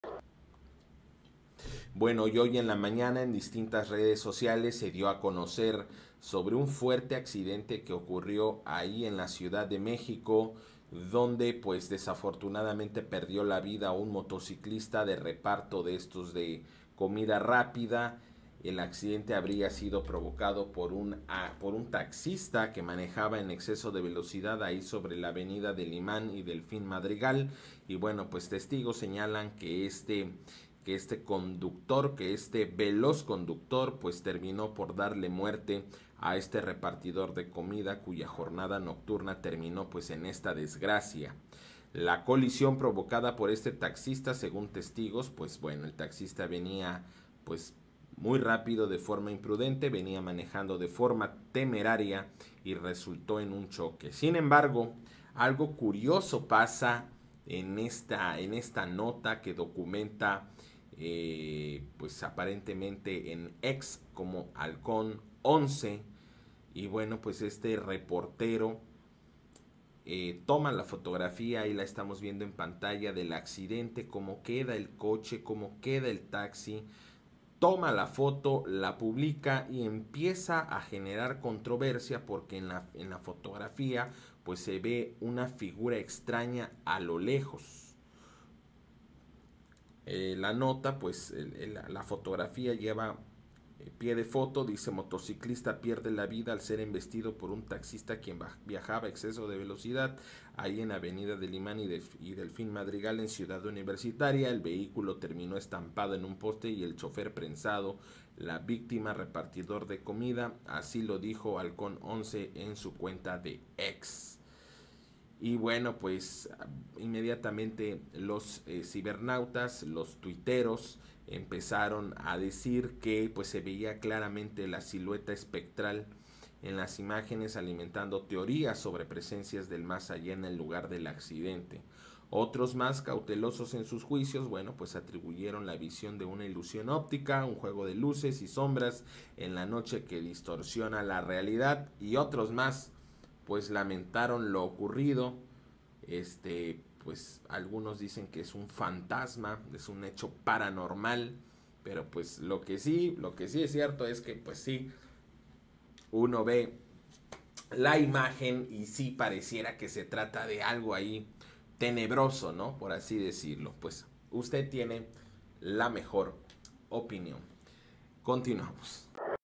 Captan extraña figura fantasmal en accidente vial en la ciudad de México ￼
Un taxista impacto de forma brusca un motociclista quitándole la vida de forma inmediata, un usuario de la red social “X” capturó una fotografía donde se logra apreciar una figura fantasmal al fondo del accidente. ￼